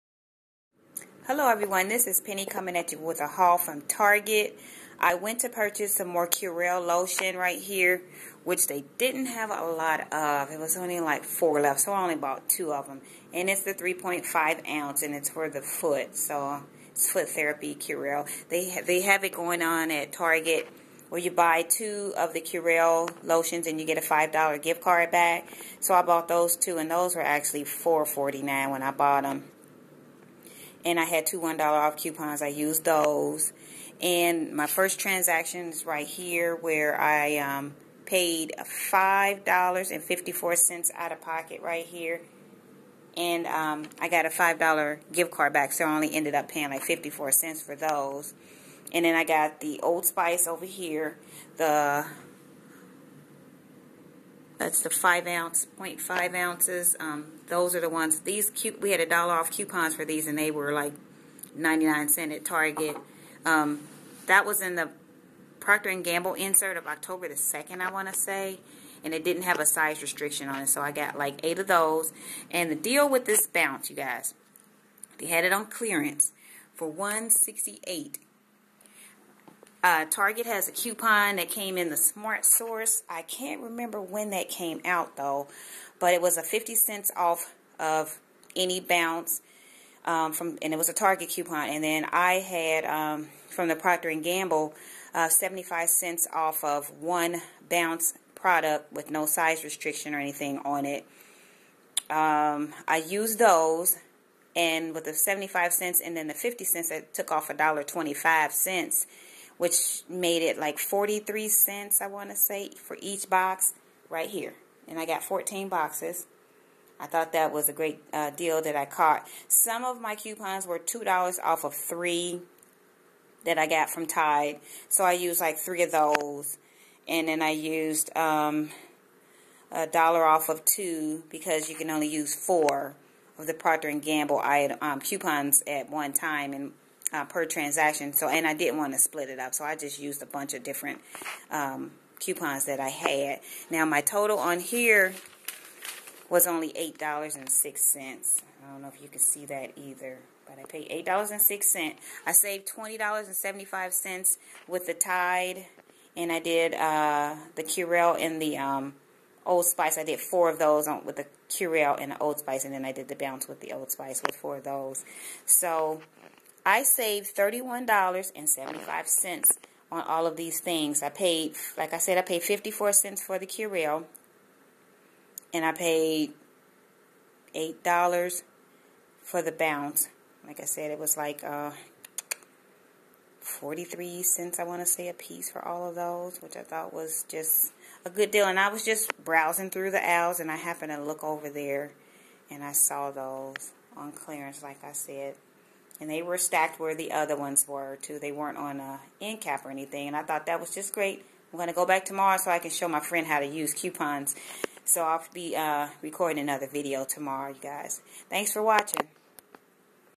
EXTREME COUPONING!!!!!!!!!! At Target- Bounce dryer sheets for 56 cents....
I used (3) 2 dollars off 3 bounce and (1) 1 dollar off 2 bounce and (5) 75/ cents off 1 and 14 target coupons
that was 14 manufacturer q's and 14 target q's making them about 56 cents a piece. The old spice was FREE! I walked out of target with a 5 dollar gift card. The Curel was $3.59 not $4.49 at my target